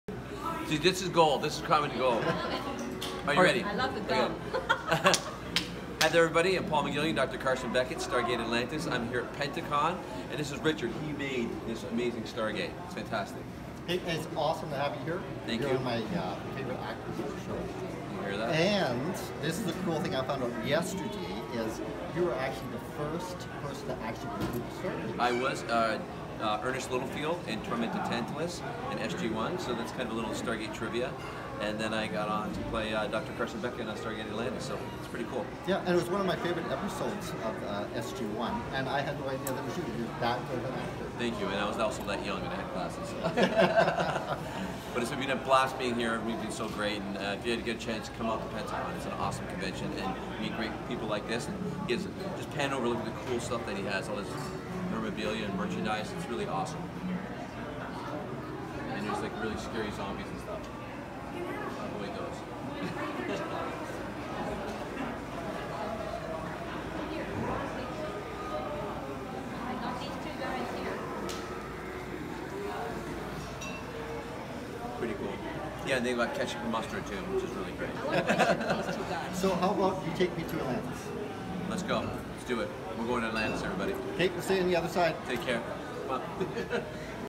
Paul McGillion from Stargate Atlantis Visit
Paul McGillion who played Dr Carson Beckett on Stargate Atlantis stopped by Empire Movie Props for a visit and to see our Stargate. He thought it was amazing .Paul is great actor and a super all around nice guy...thanks Paul 🙂